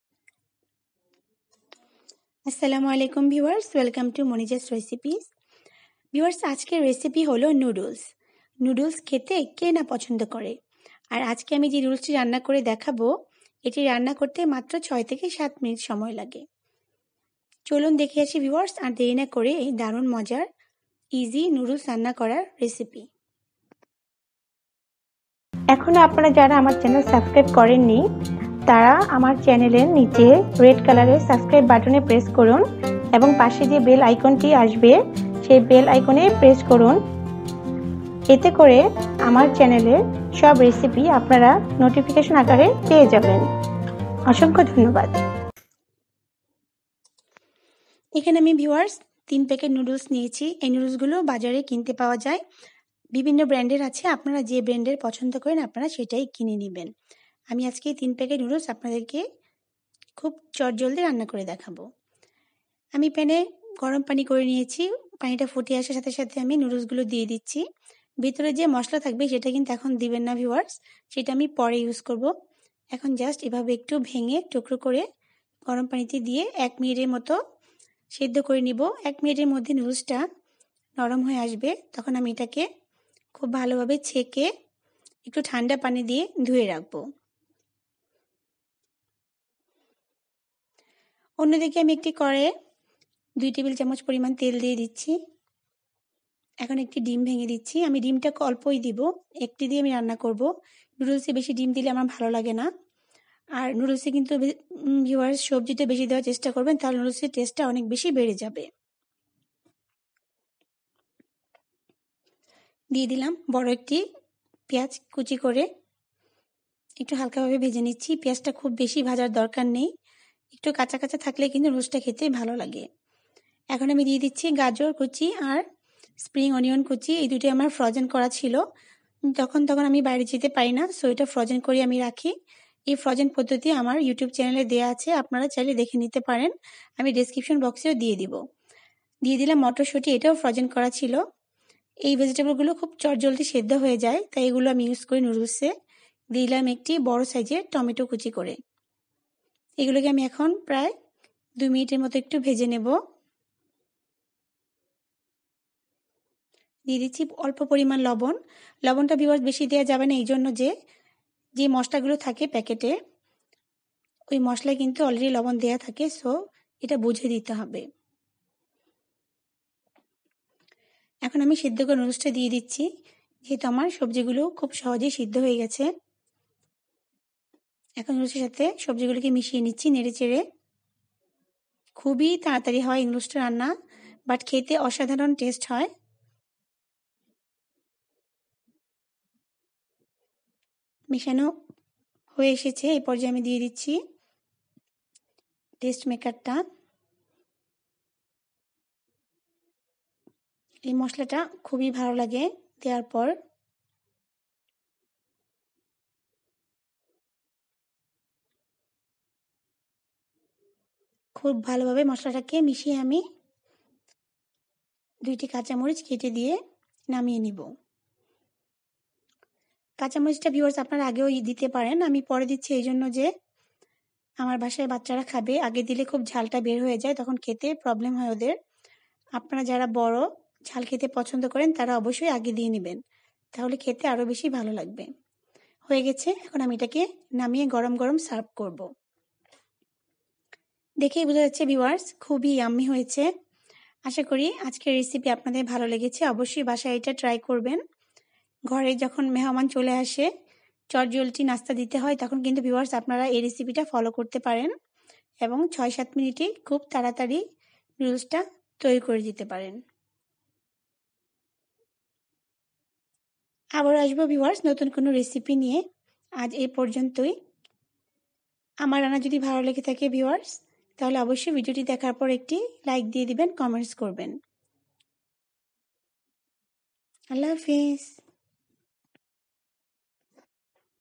ভেজিটেবল এগ নুডুলস# দারুন স্বাদের নুডুলস্ ৭ মিনিটে তৈরীTasty Vegetable Egg Noodles Noodles recipe
রেসিপির নাম : ভেজিটেবল এগ নুডুলস
প্রকাশের তারিখ : ২১ ফেব্রুয়ারী ২০১৯
সময় : সন্ধা ১০টা ৩৬ মিনিটে